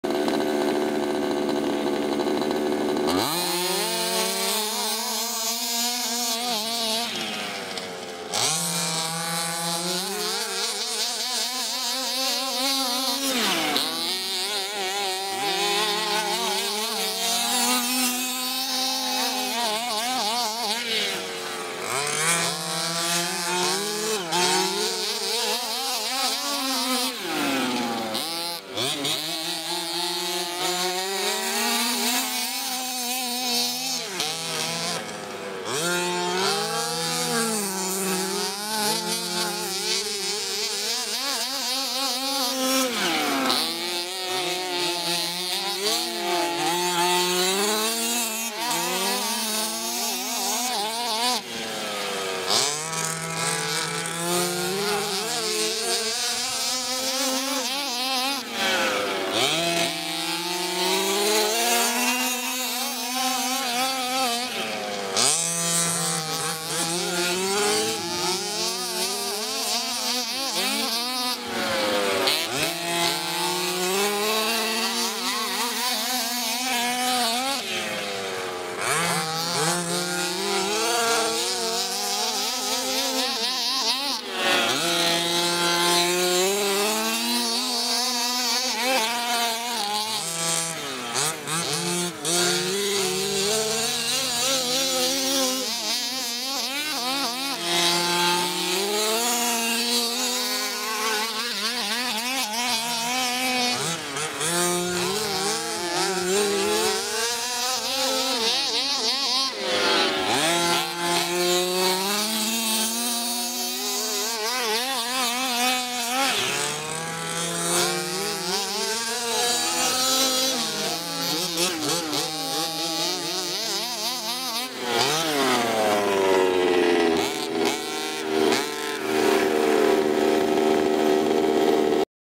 hpi bajas racing,p. 2 of 4
Heres two bajas racing at south beach green arklow IRL. this video was taken on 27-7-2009